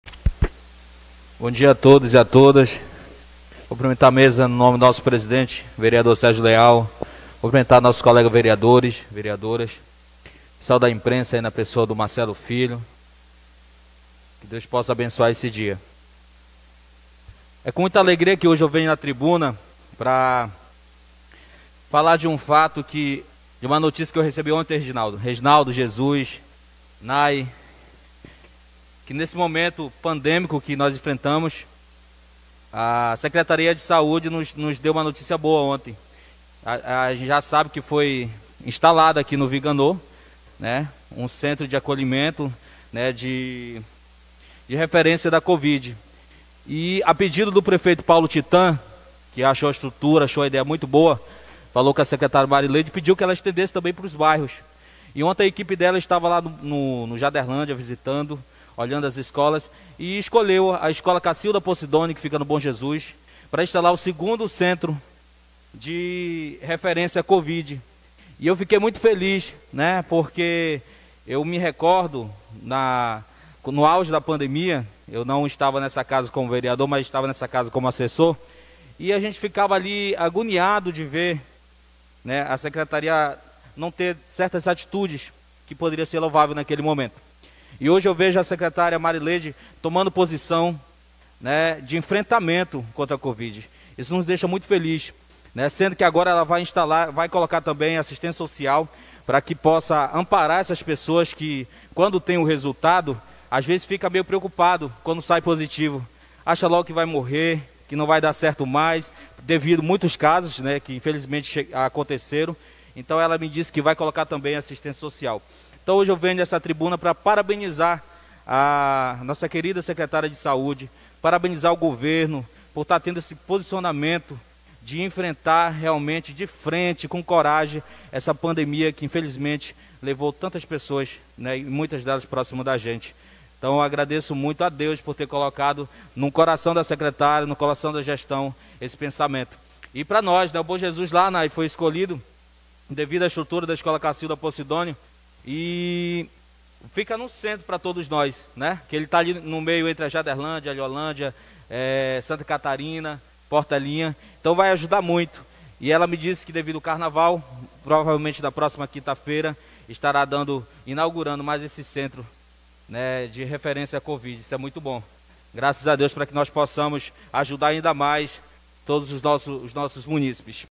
centro referencial da covid
Chegamos a mais um fim  de semana ,mas já estamos com  a cabeça na próxima semana ,onde deverá ser inaugurado mais um centro de referência a covid em Castanhal , específicamente no bairro Bom Jesus ,na escola Cacilda Possidônio.
E com muita alegria demos essa notícia,pois entendemos da importância desse centro em combater a covid e ajudar salvar vidas .